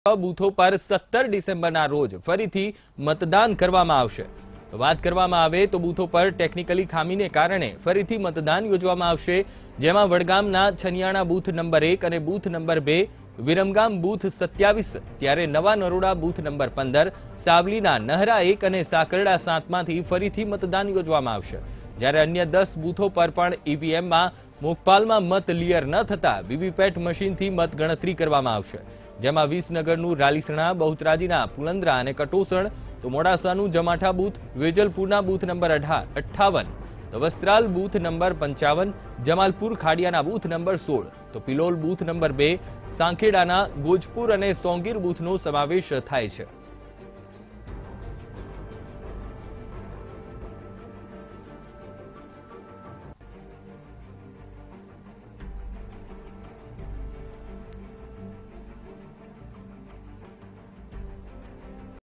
Gujarat Elections: Re-elections will be carried out on few booths on 17th December | Vtv News
Gujarat Elections: Re-elections will be carried out on few booths on 17th December